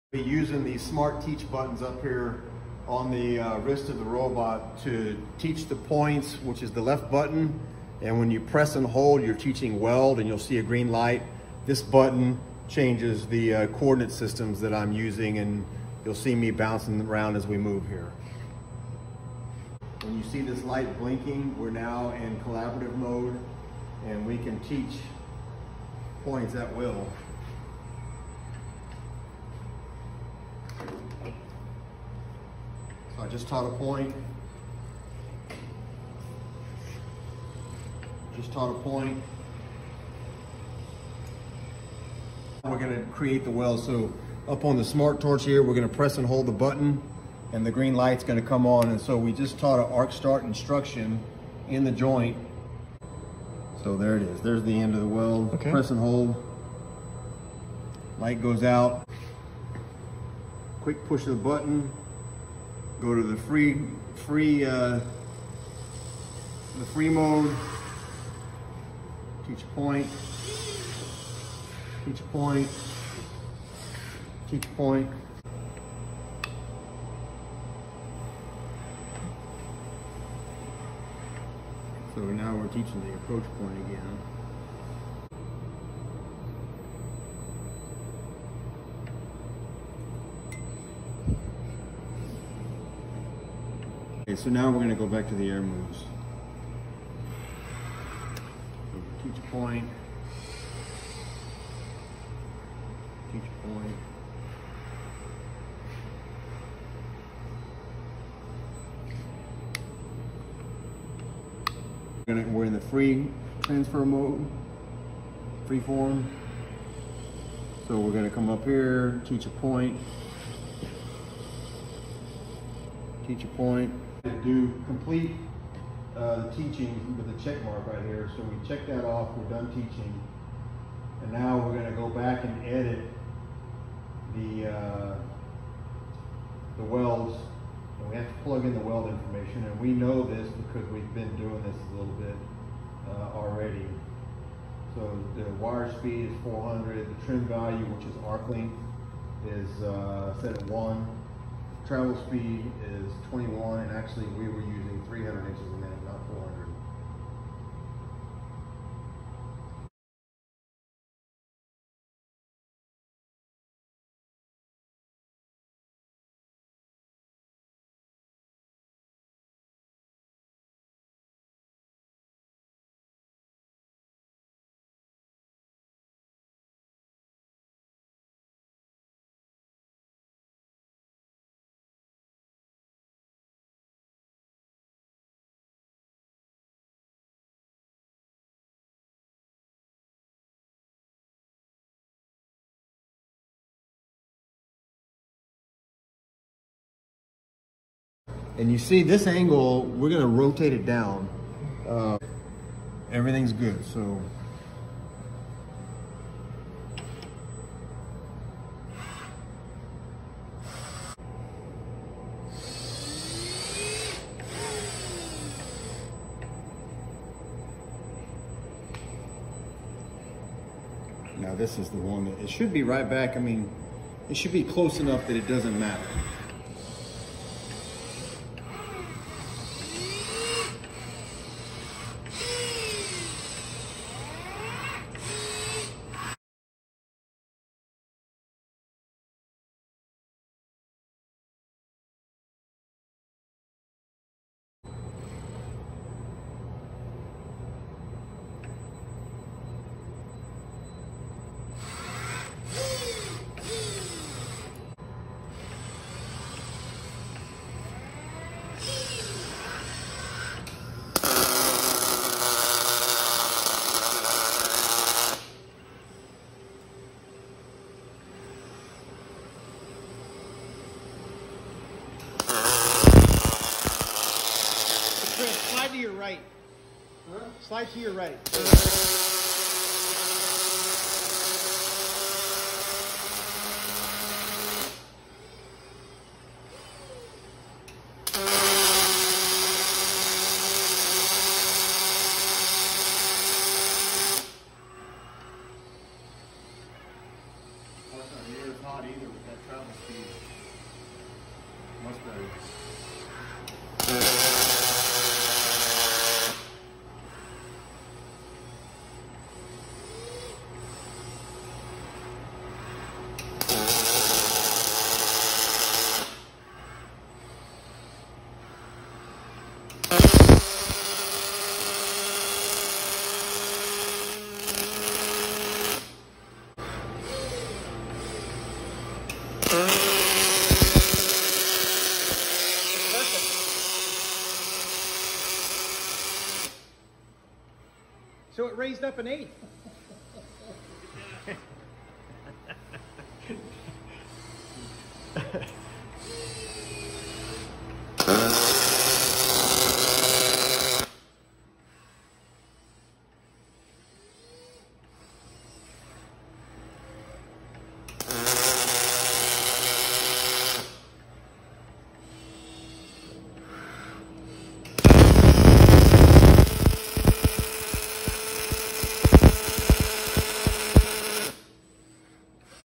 Lincoln Cobot 11 17 21
Initial trials learning the Lincoln Fanuc Cobot.